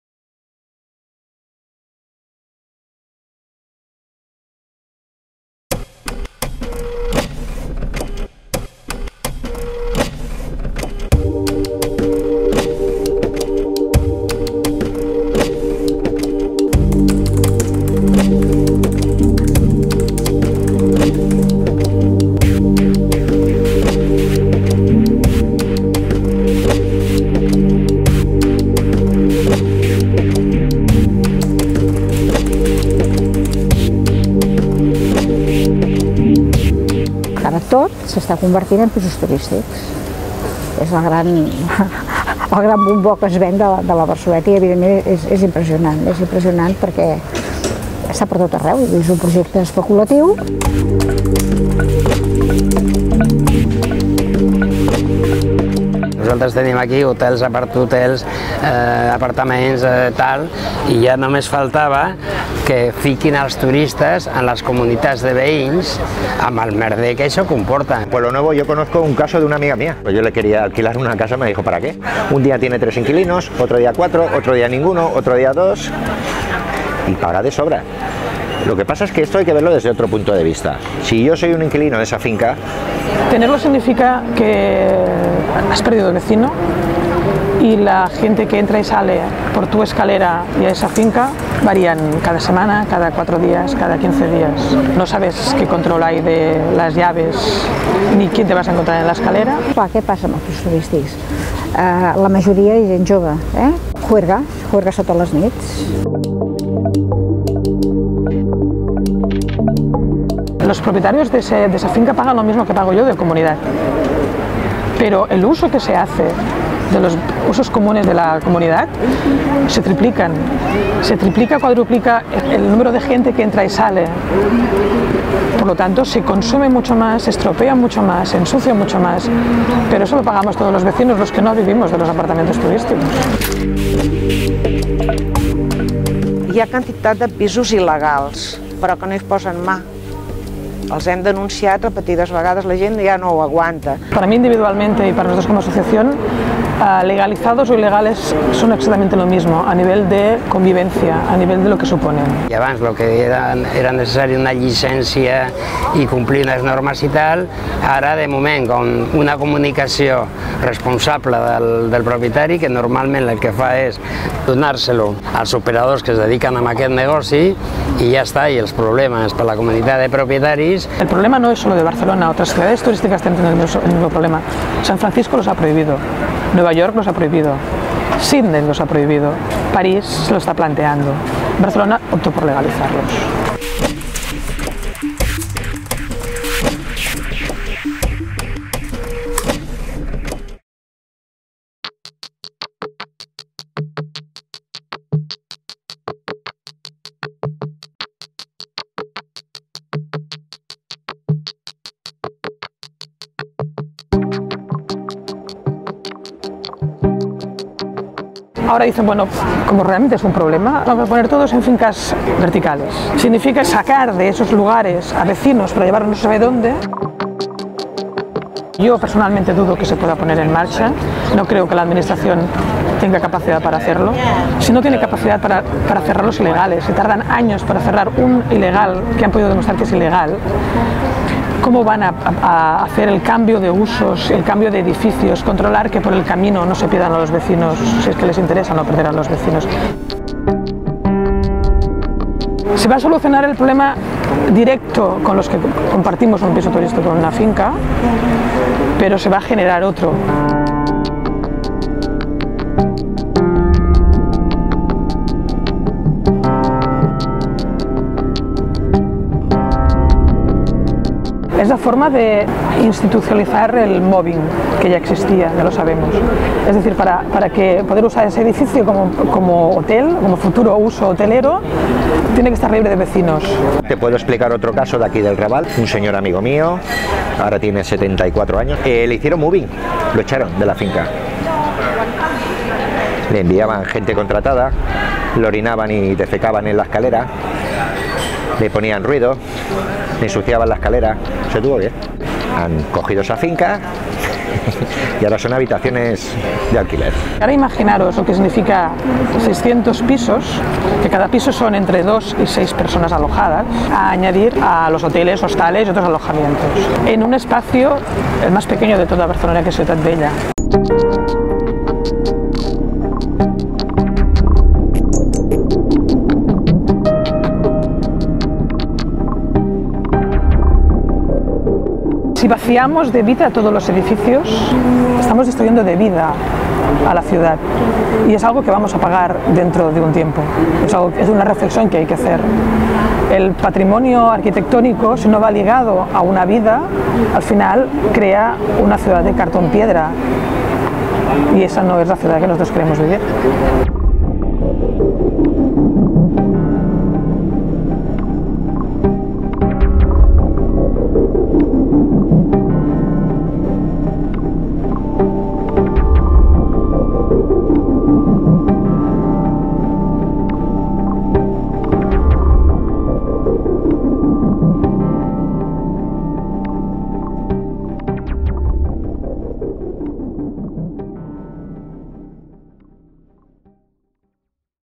Tourist flats Barcelona
///English & Spanish descriptions///
In Barcelona tourists can now feel at home. It's no longer necessary to stay in a hotel or a hostel, you can rent by days cheap apartments in residential buildings, and share the joy of Barcelona with it's neighbors.

En Barcelona es posible que los turistas se sientan como en casa. Ya no es necesario quedarse en hoteles u hostales porque ahora puedes alquilar por días apartamentos baratos en edificios residenciales y así sentir la alegría de Barcelona junto a sus vecinos.